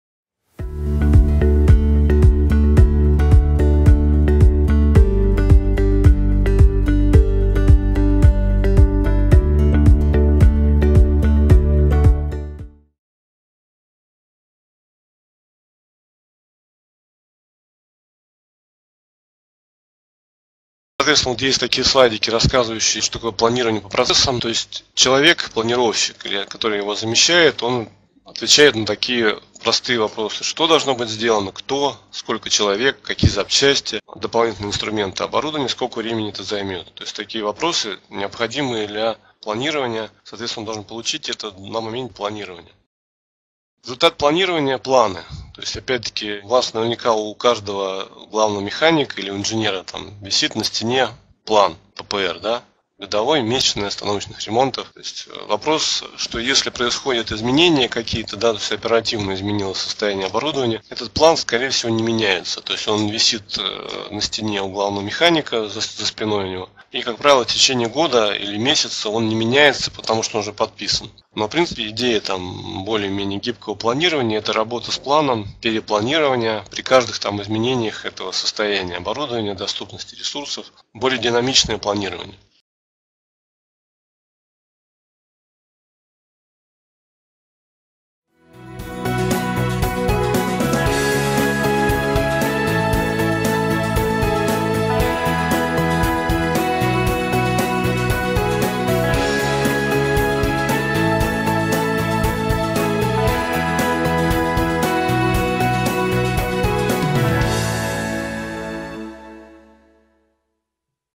Планирование по процессам. Результат планирования — ПЛАН. ТОиР. RCM
Тема сегодняшнего видео «Планирование по процессам. Результат планирования — ПЛАН. ТОиР. RCM».

00:00 prostoev.net
00:13 В предыдущем видео мы говорили про горизонты планирования ТОиР
00:21 Планирование по процессам
00:43 Результат планирования - ПЛАН
01:23 В следующем видео: система заказ-нарядов работ.Work order
01:28 Приходите на наш курс «Базовые практики планирования ТОиР, управления надежностью и критичностью оборудования по методике RCM»